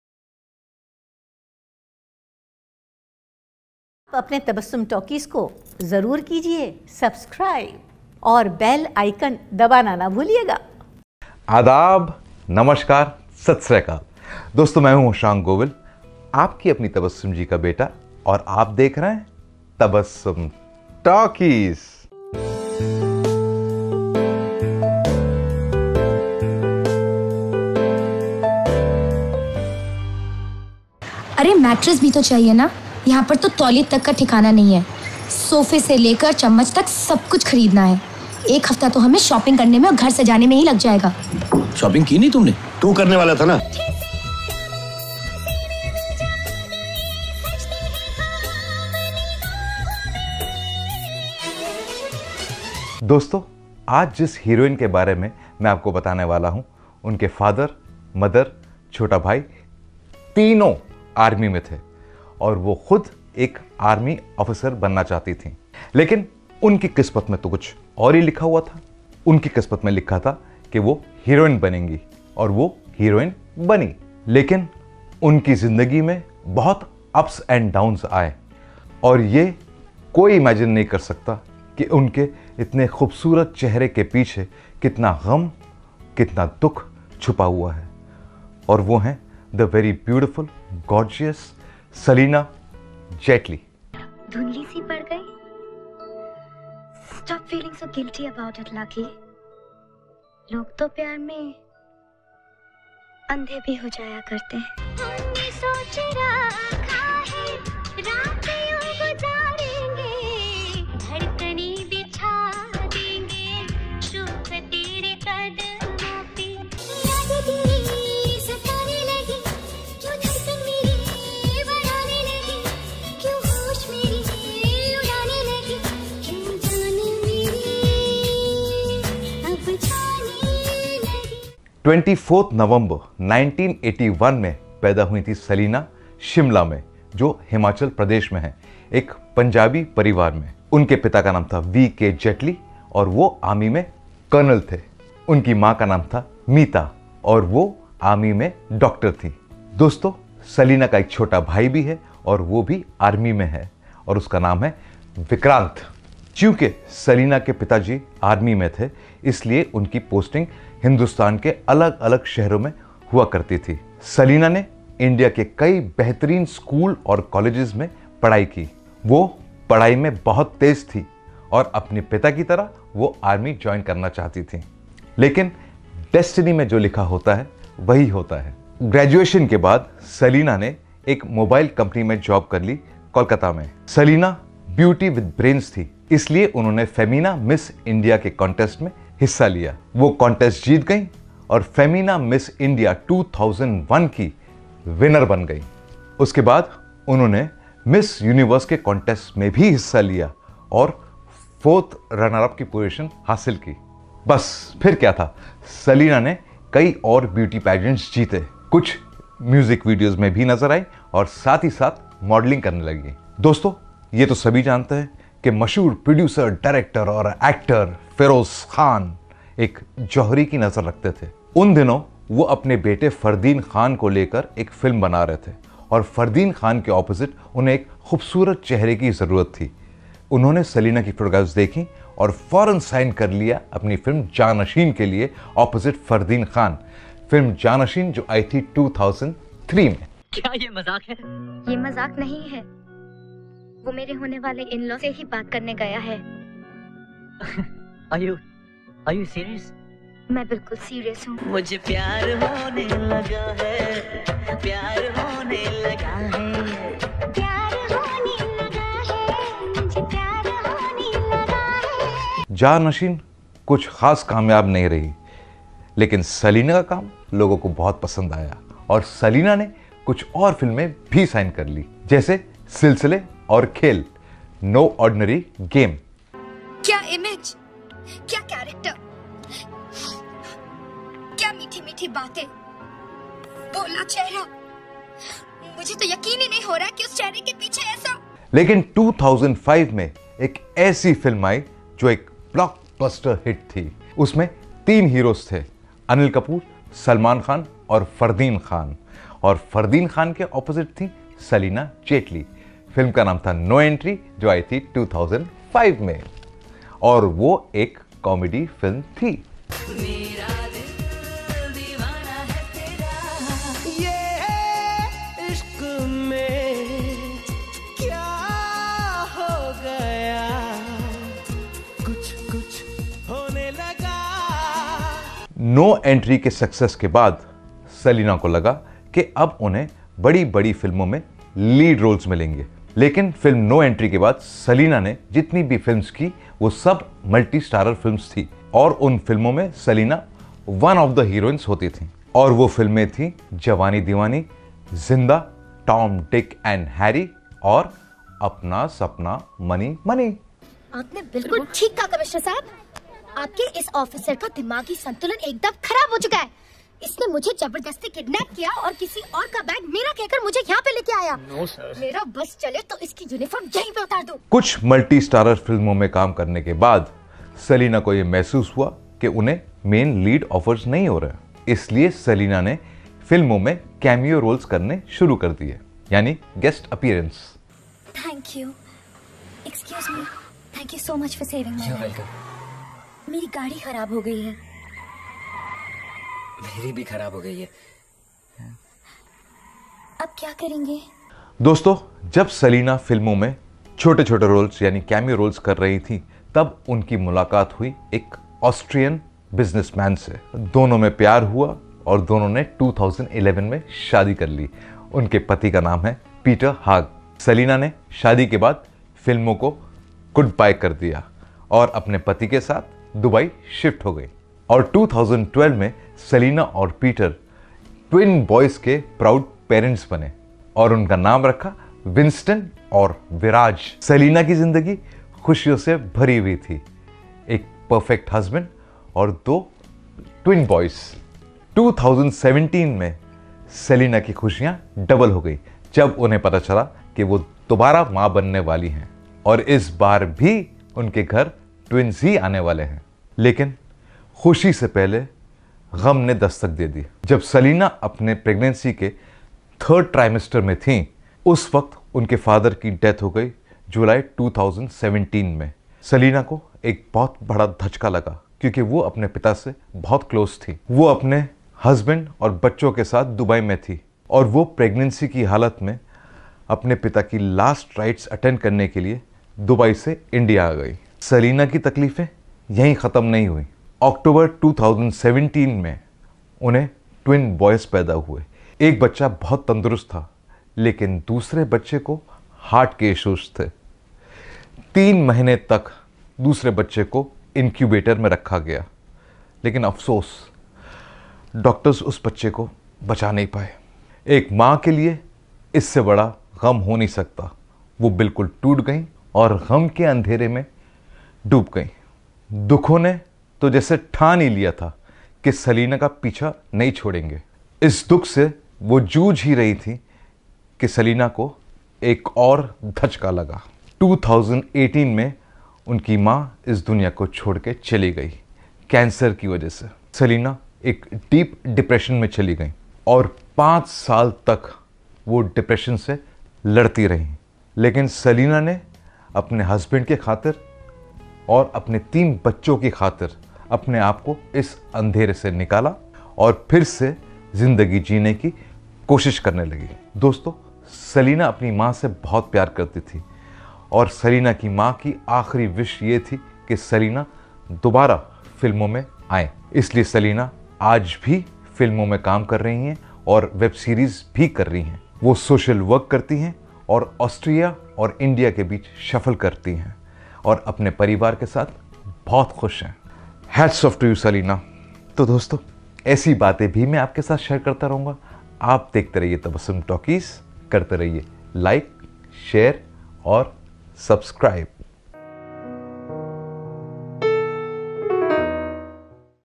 Celina Jaitly: The Actress Who Wanted To Join The Army | Tabassum Talkies
Celina Jaitly was a prominent actress in the early 2000's who unfortunately couldn't get the fame. She soon quit Bollywood and started a family with her Austrian husband in Dubai, however in 2017 a series of setbacks and personal loss left her in depression. Here's the story of Celina Jaitly!